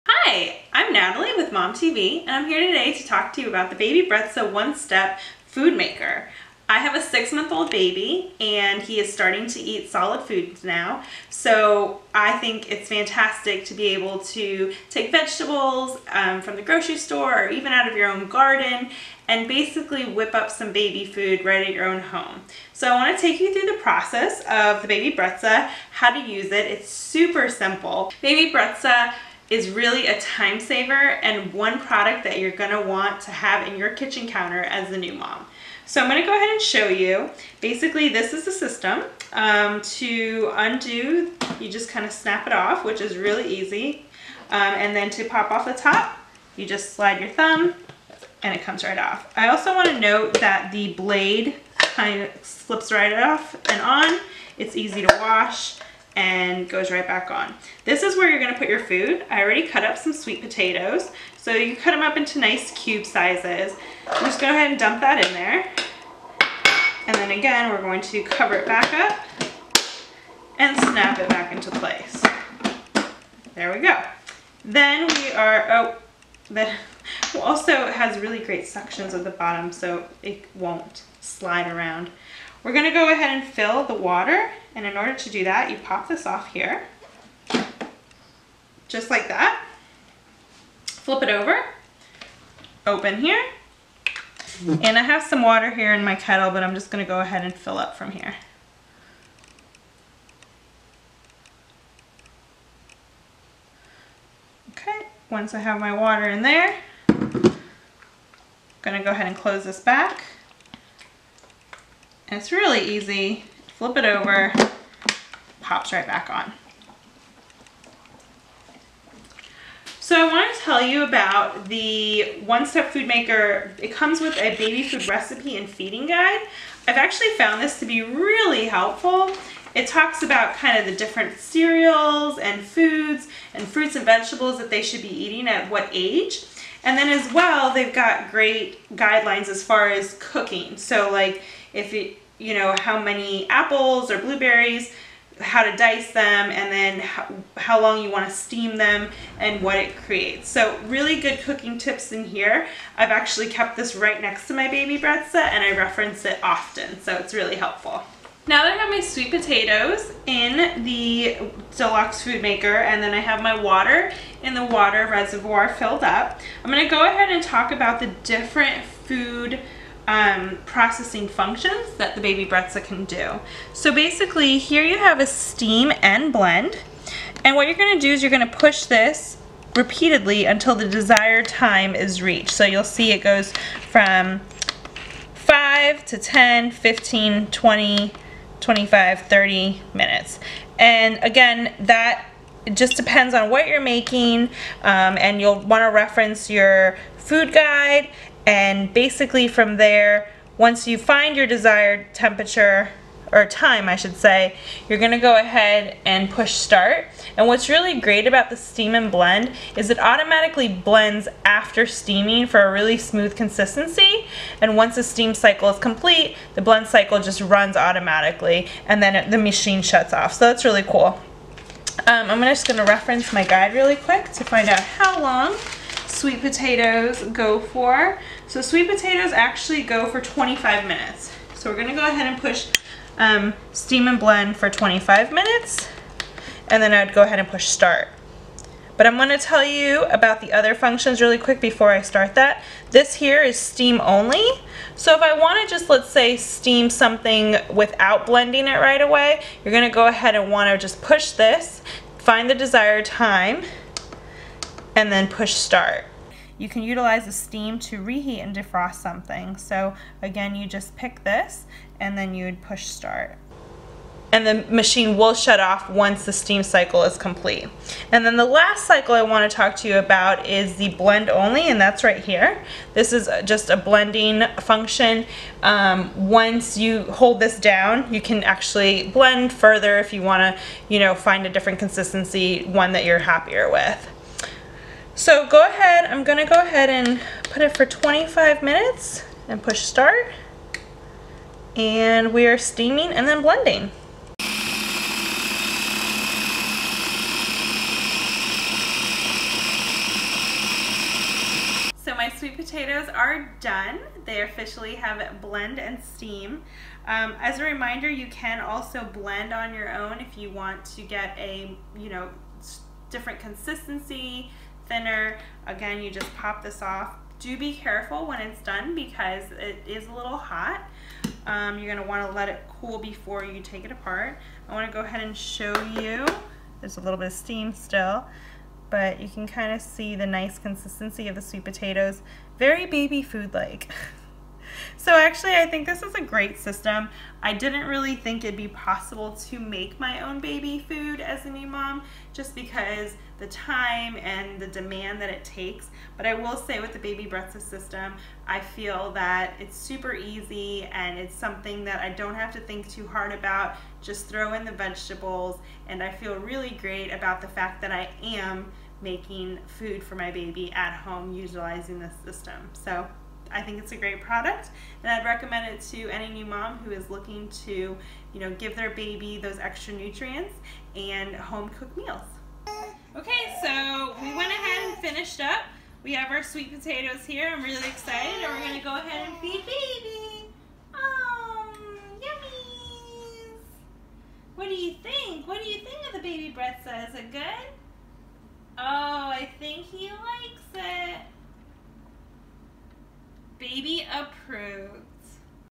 Making Baby Food at Home with the Baby Brezza One Step Baby Food Maker Deluxe
I've sworn by Baby Brezza products for years! They have been a client of mine, but I bought a Baby Brezza Baby Food Maker for my friend Natalie. In this video she shares her thoughts and opinions on the Baby Brezza One Step Baby Food Maker Deluxe.

Making your own homemade baby food is convenient and easy with the Baby Brezza One Step Baby Food Maker Deluxe. This appliance automatically steams and blends food with the push of a button in one bowl without requiring any transfer of hot food. It as little as 10 minutes, you can make homemade baby food which is better for your baby and can save you hundreds of dollars vs store-bought.